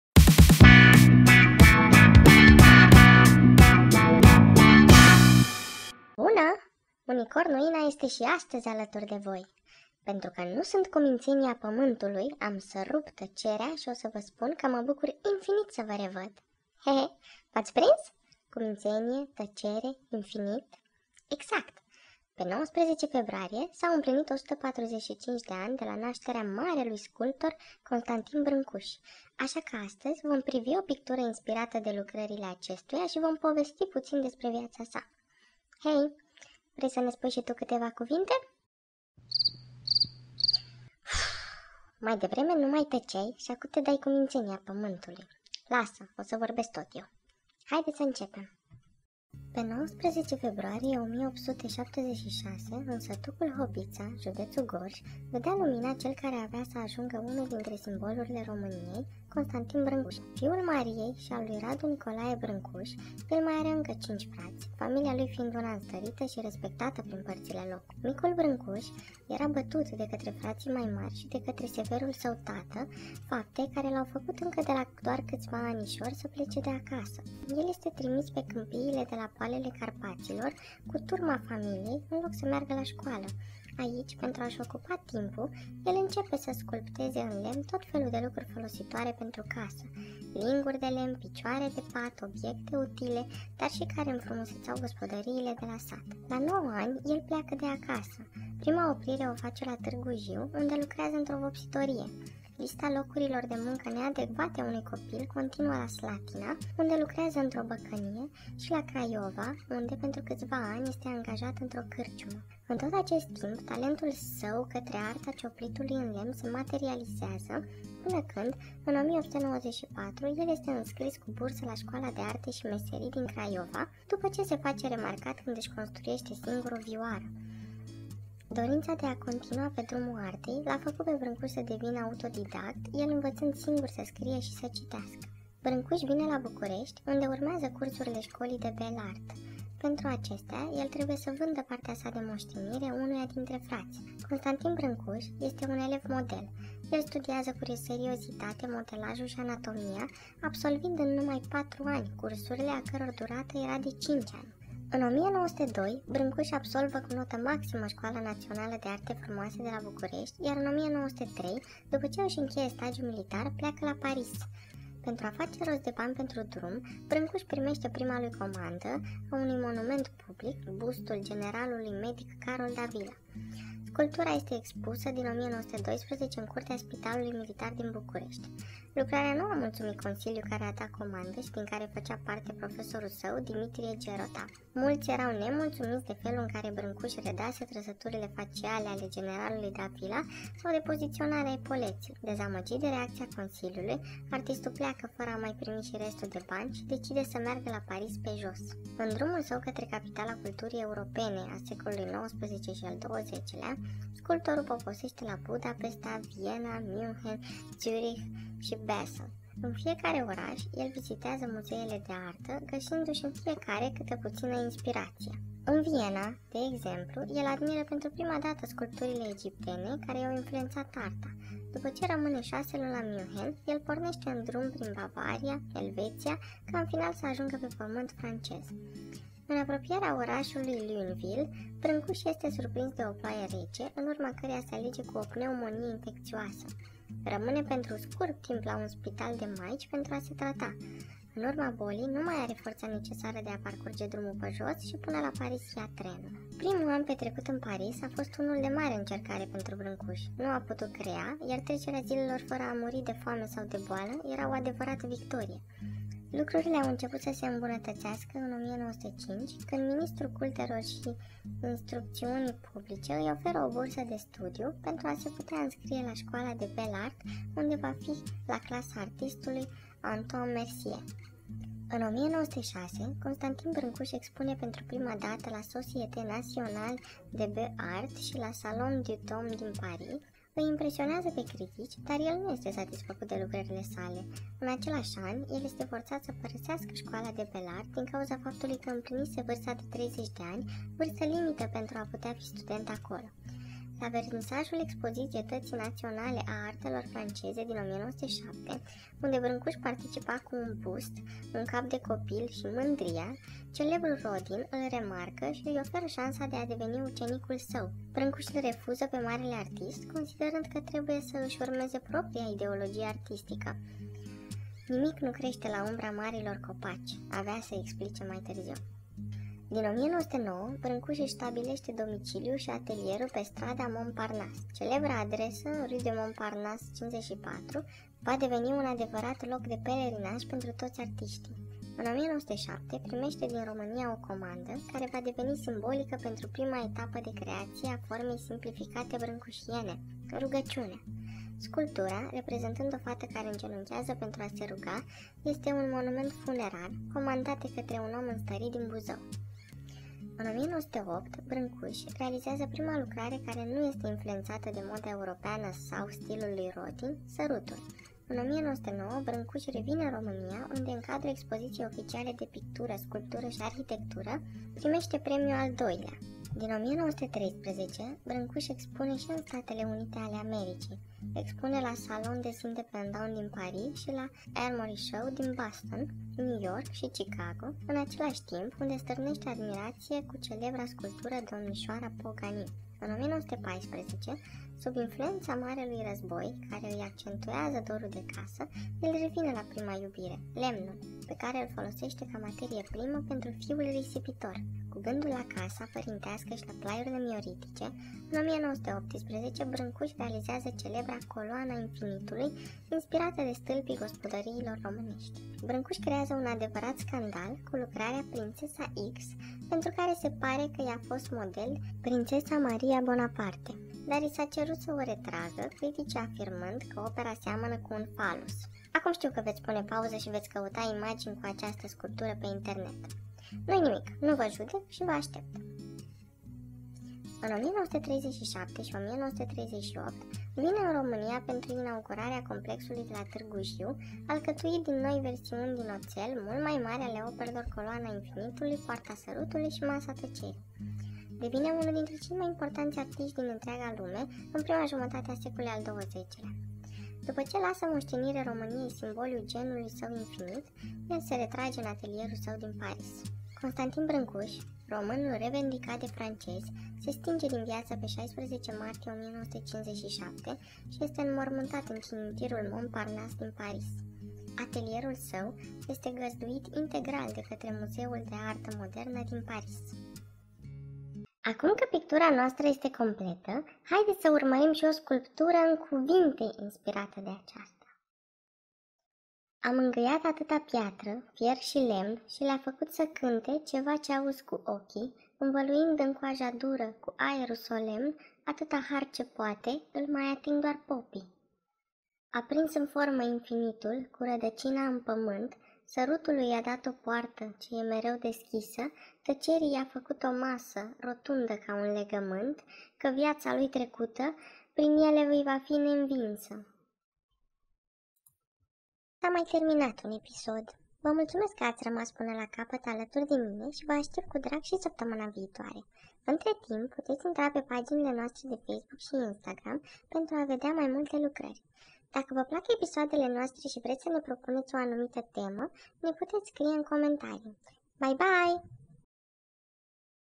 ART Blender- Episodul 3. Constantin Brancusi. Pictura-time lapse (acrilic+soft pastel )
Pictura in acrilic si soft pastel pe hartie A4- time lapse. Poveste- Viata lui Constantin Brancusi .